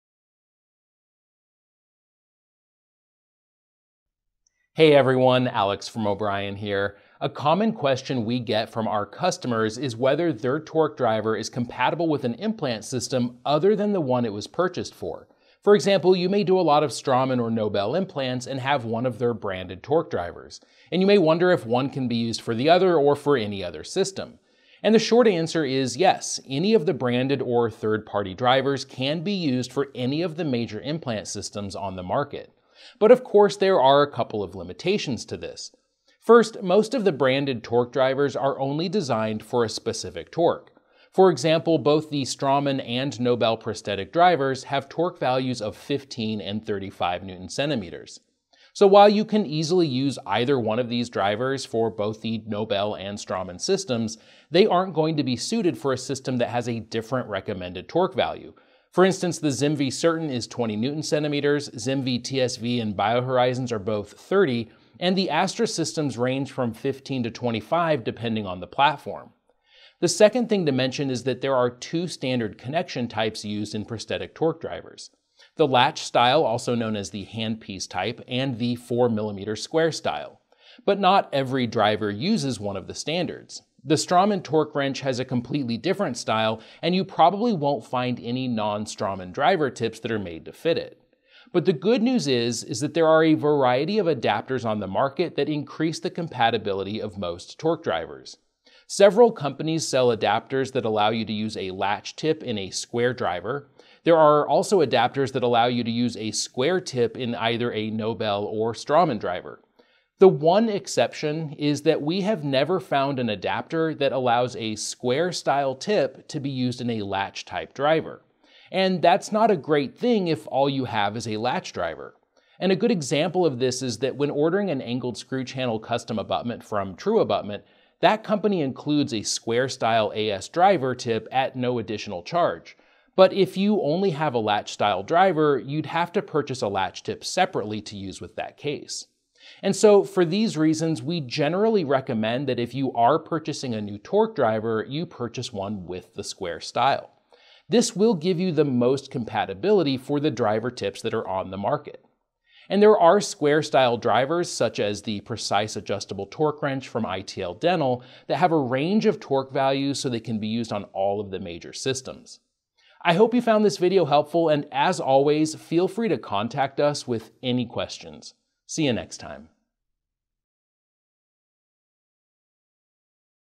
Torque Driver Compatibility
In this video, Alex discusses the compatibility of dental prosthetic torque drivers.

00:00 Is it compatible?

A common question we get from our customers is whether their torque driver is compatible with an implant system other than the one it was purchased for.

For example, you may do a lot of Straumann or Nobel implants and have one of their branded torque drivers.  And you may wonder if one can be used for the other, or for any other system.

Yes, any of the branded or 3rd party drivers can be used for any of the major implant systems on the market - but there are limitations.

00:33 Limited Torque Values

Most of the branded torque drivers are only designed for a specific torque.  For example, both the Straumann and Nobel prosthetic drivers have torque values of 15 and 35 Ncm.  So, while you can easily use either one of these drivers for both the Nobel and Straumann system, they aren't going to be suited for a system that has a different recommended torque value.

For instance, the ZimVie Certain is 20 Ncm, ZimVie TSV and BioHorizons are both 30, and the Astra systems range from 15 to 25 depending on the platform.

01:10 Standard Connections
There are two standard connection types used in prosthetic torque drivers.  The latch style, also known as the handpiece type, and the 4mm square style.  But not every driver uses one of the standards.  The Straumann torque wrench has a completely different style and you probably won't find any non-Straumann driver tips that are made to fit it.

01:31 Connection Adapters
There are a variety of adapters on the market that increase the compatibility of most torque drivers.

Several companies sell adapters that allow you to use a latch tip in a square driver.  And there are also adapters that allow you to use a square tip in either a Nobel or Straumann driver.

The one exception is that we have never found an adapter that allows a square style tip to be used in a latch type driver.  And that's not a great thing if all you have is a latch driver.  A good example of this is that when ordering an Angled Screw channel custom abutment from TruAbutment, that company includes a square style AS driver tip at no additional charge.  But if you only have a latch style driver, you would have to purchase a latch tip separately.

02:20 Our Recommendation

For these reasons, we generally recommend that if you are purchasing a new torque driver you purchase one with the square style.  This will give you the most compatibility for the driver tips that are on the market.

There are square style drivers such as the Precise Adjustable torque wrench from ITL Dental that have a range of torque values so they can be used on all of the major systems.

Alex Rugh, CDT
O'Brien Dental Lab